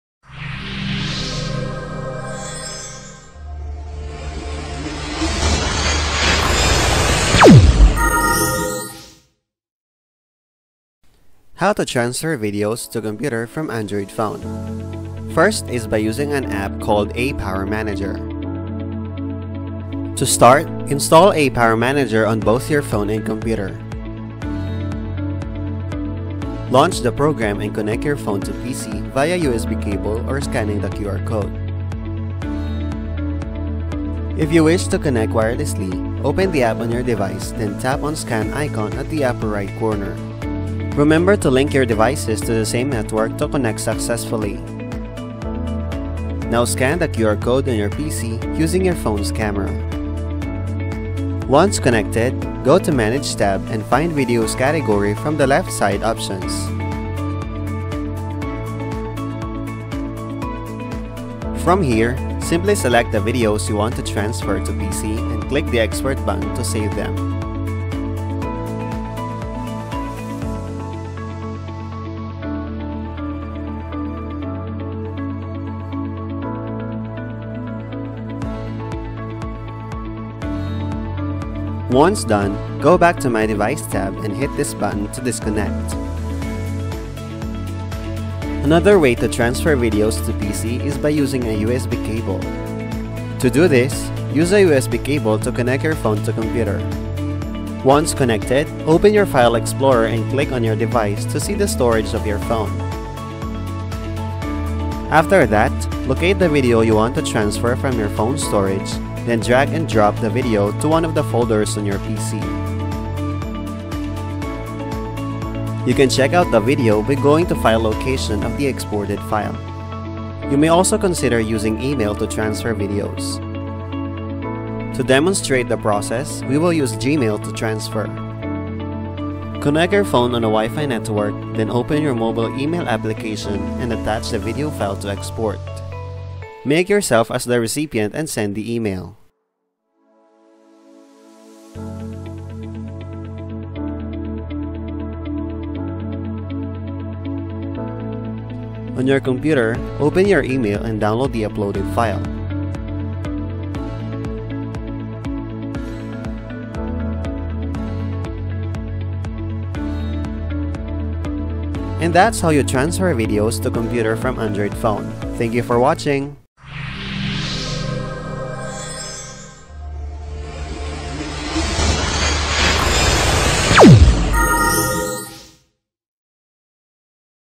How to Transfer Videos to Computer from Android Phone
To avoid stacking up tons of video files on your phone, it is imperative to transfer videos from Android to computer. ApowerManager is a good tool for you to do this. Watch this video to learn more.